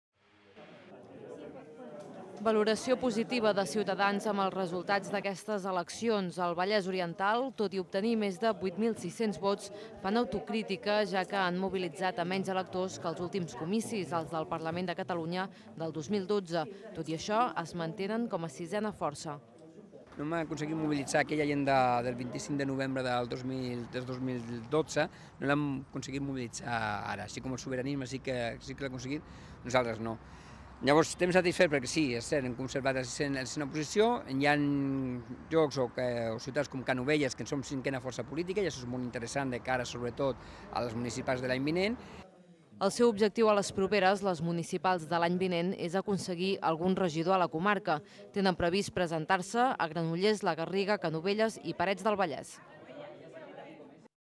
VOTV - Ciutadans mobilitza menys electors vallesans del que esperaven
Notícia emesa a l'informatiu el dia 26 de maig de 2014.